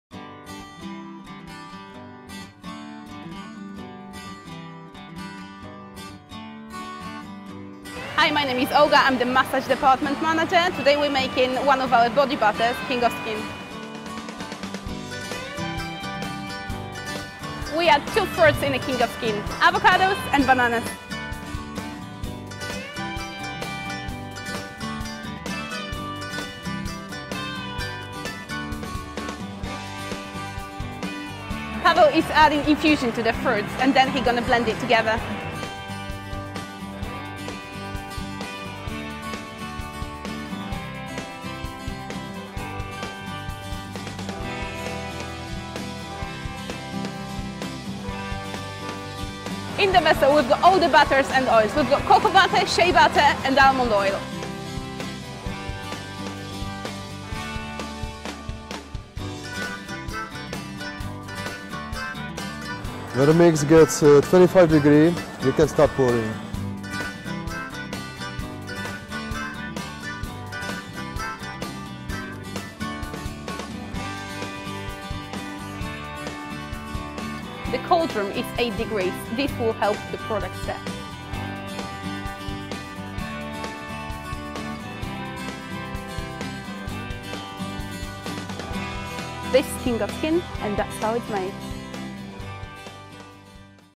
Lush presenta: Balsamo Corpo Parole di Burro
Balsamo irresistibilmente ricco, per nutrire la pelle golosa e assetata di idratazione con il massimo della morbidezza.